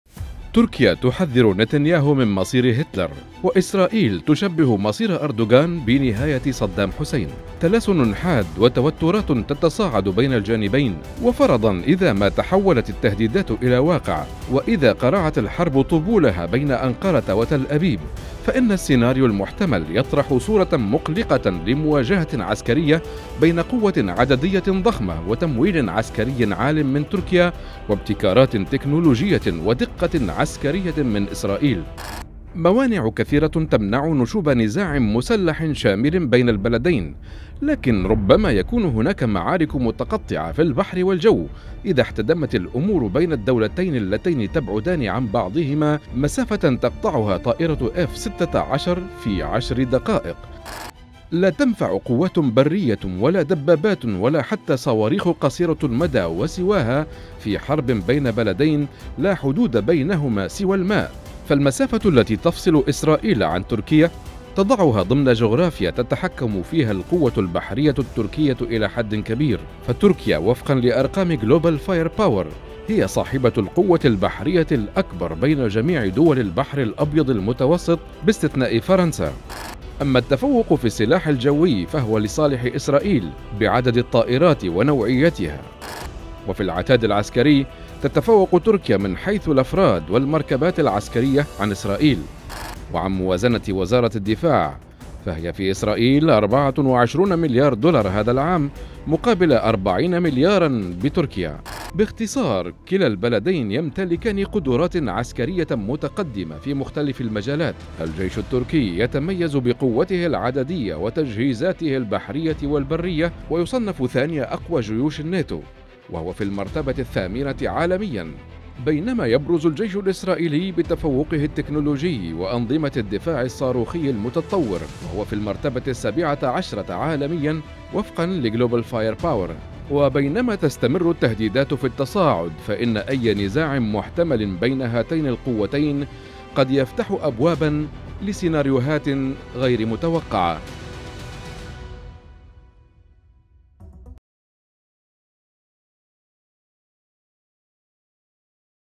ماذا لو أعطى أردوغان الأمر بالهجوم على إسرائيل؟
"فرضا إذا قرعت الحرب طبولها بين أنقرة وتل أبيب.. كيف سيكون شكلها بين بلدين لا حدود بينهما سوى الماء؟"